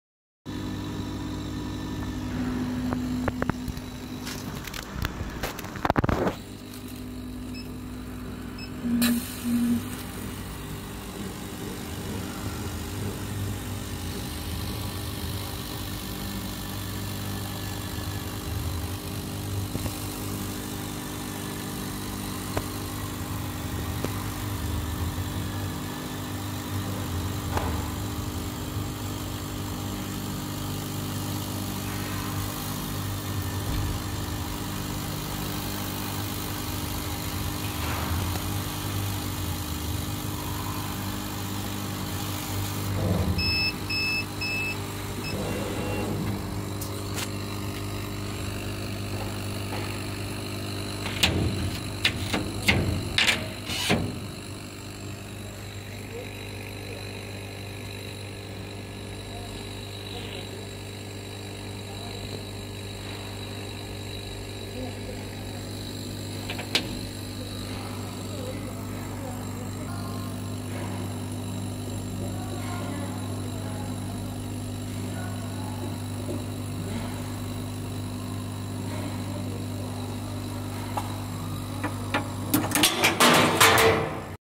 To Dan W4 Co2 Laser Cutter Machine to Cut 9.2mm Acrylic
130W Reci W4(100W-130W) Co2 Laser Tube Engraver Cutter Machine with CW5000 S&A Water Chiller Laser Engraving Machine Cutting 1300x 900mm(52"x36")

Any questions,pls feel free to let me know.